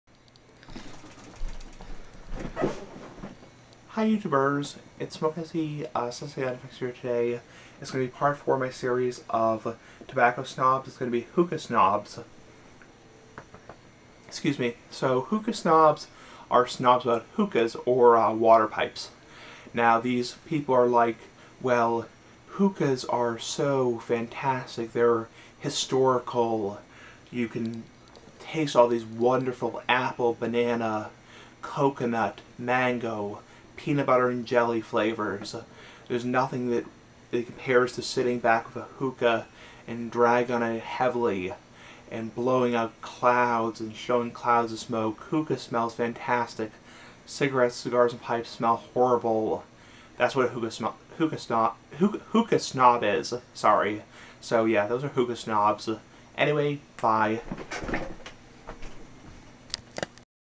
Part 4 Hookah Snobs
In this video I discuss hookah snobs.